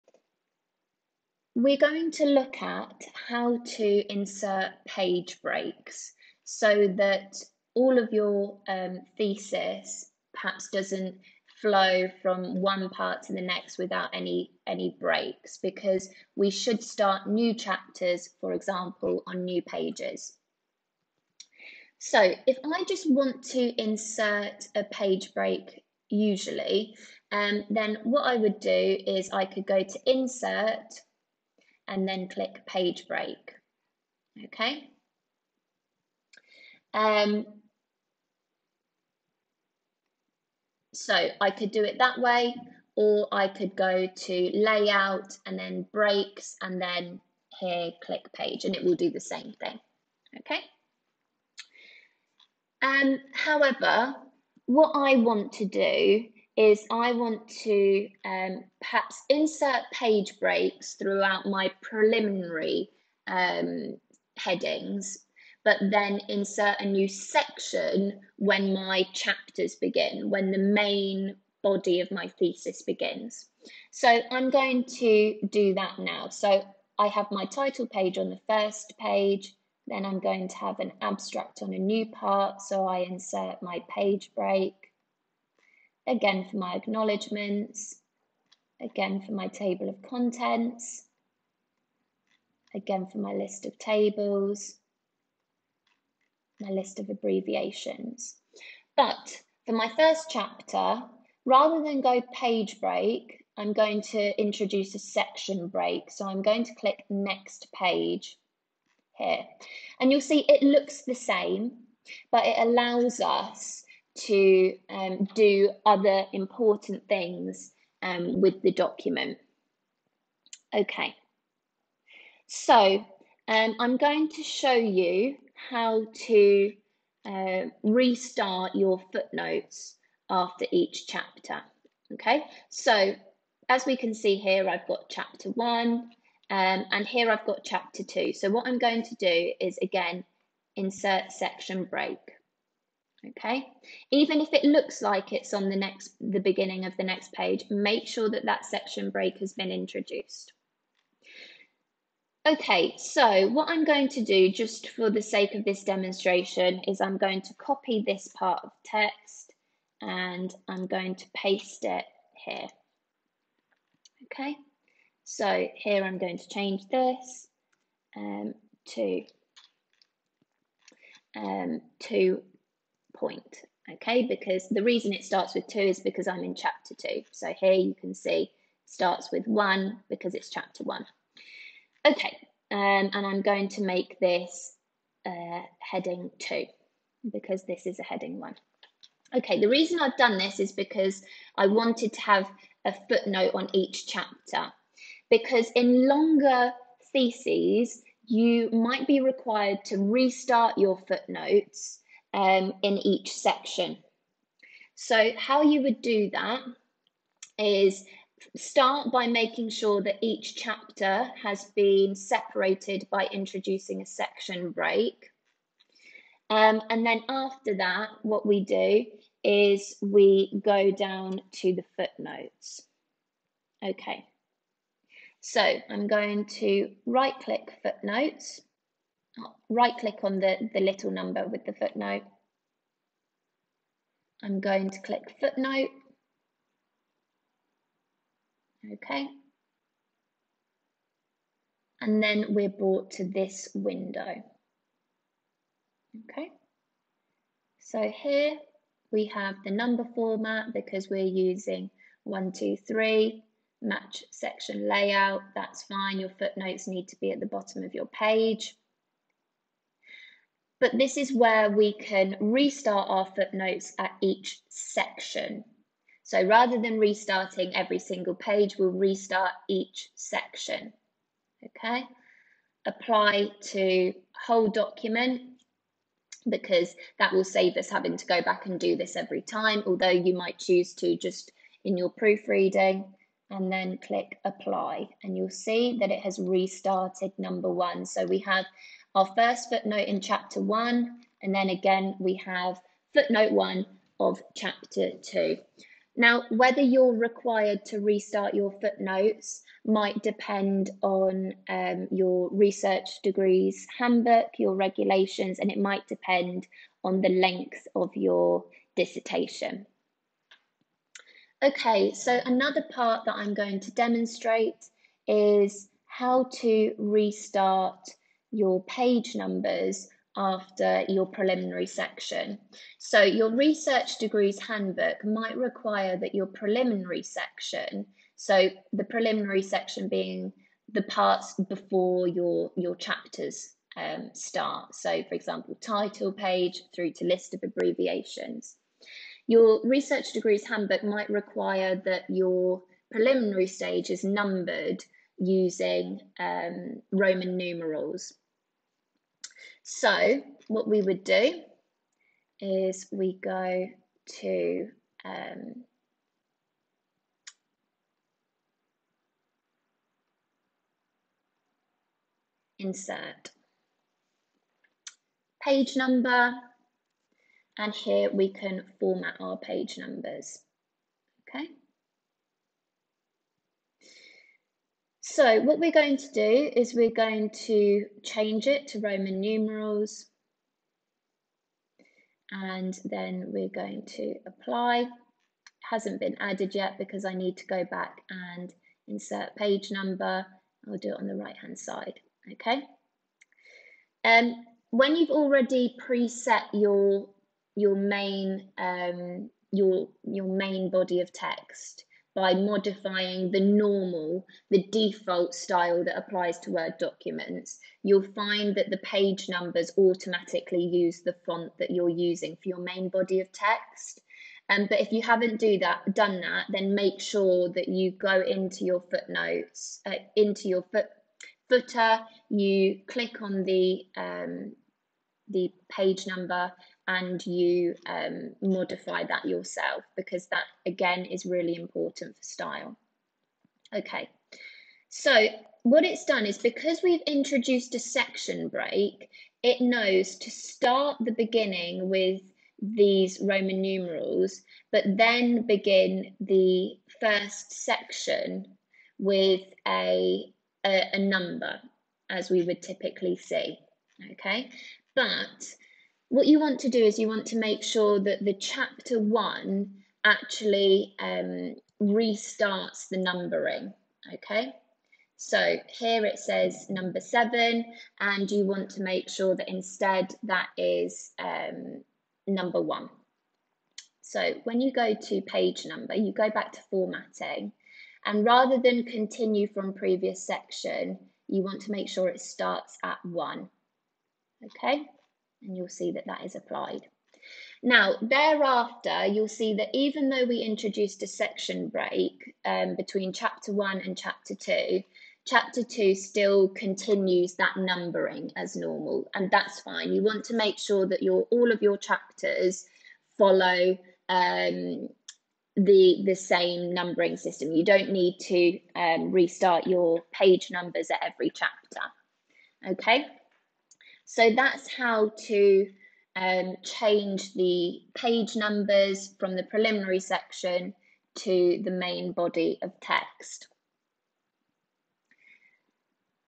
How to Insert and Modify Page Numbers and Page/Section Breaks (Microsoft Word)
00:01 How to Insert Page Break
01:40 How to Insert Section Break
03:15 How to Restart Footnotes for Each Section
05:11 How to Restart Page Numbers

What is the difference between inserting a Page Break and a Section Break? Inserting a Section Break will allow you to reset the footnotes for each section! (So, dissertation writers: Page Breaks might be more useful for preliminary material, where the page numbers are continuous throughout, but Section Breaks will be more useful for your chapters, if each chapter requires the footnotes to restart!)

Recorded January 2022 using Microsoft Word version 16.54. Accuracy of the content is subject to Microsoft updates.

You can sign up for a free Microsoft student account

Please contact IT if you have IT-related issues.